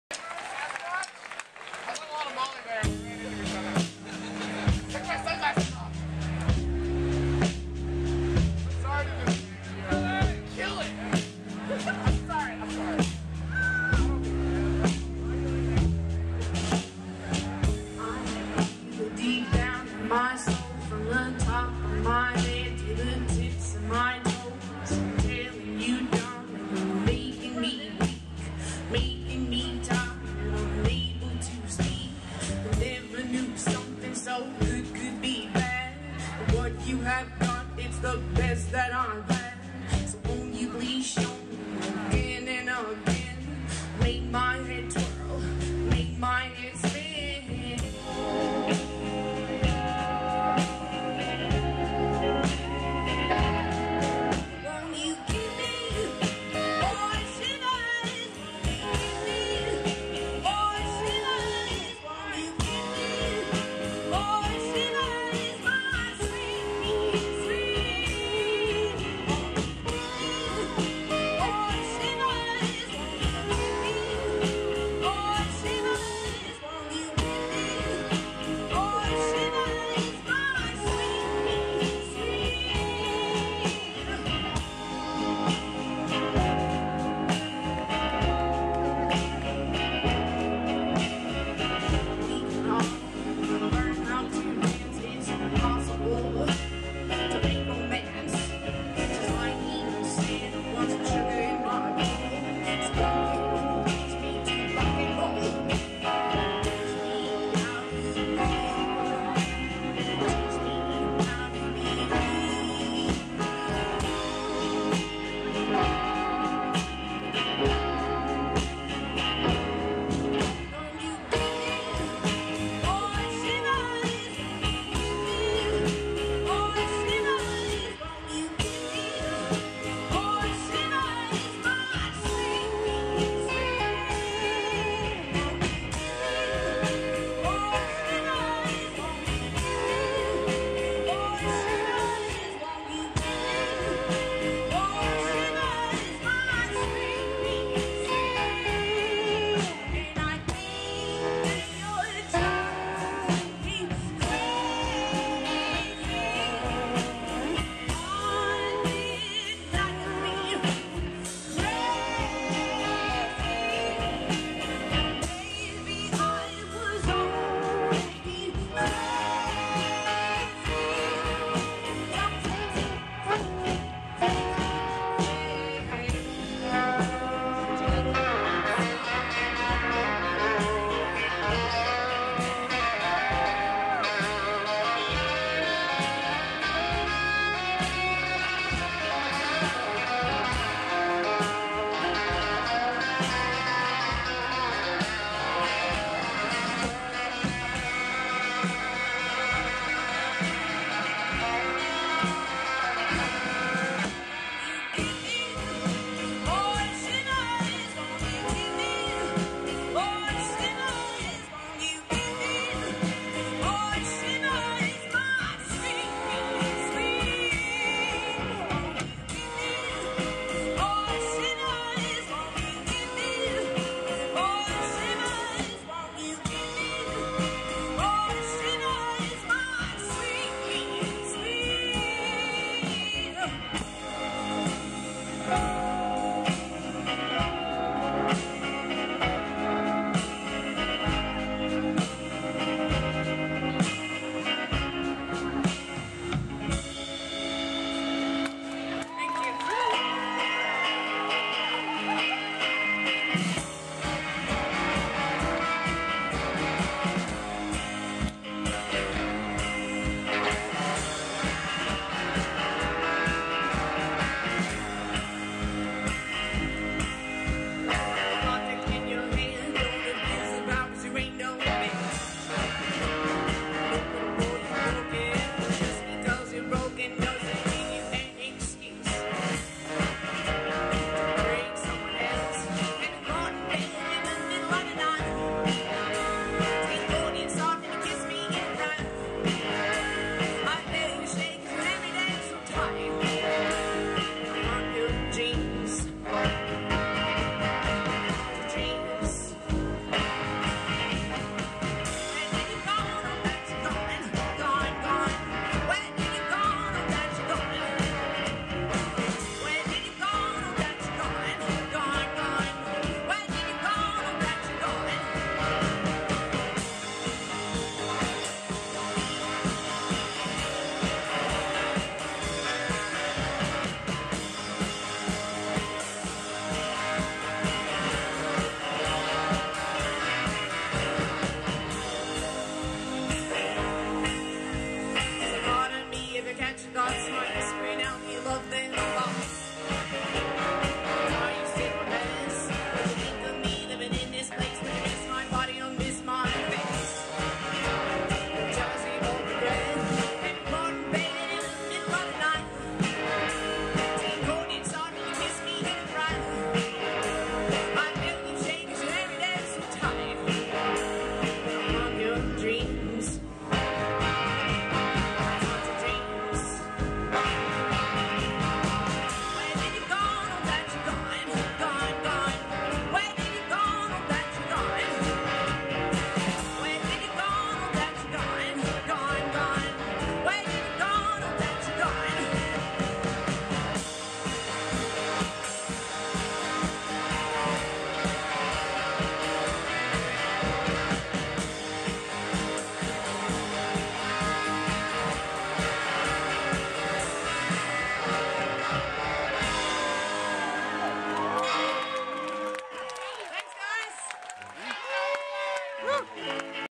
Sallie Ford and the Sound Outside play the Mural Amphitheater
Sallie Ford and the Sound Outside play the free No Depression show at the Mural Amphitheater on August 3, 2012.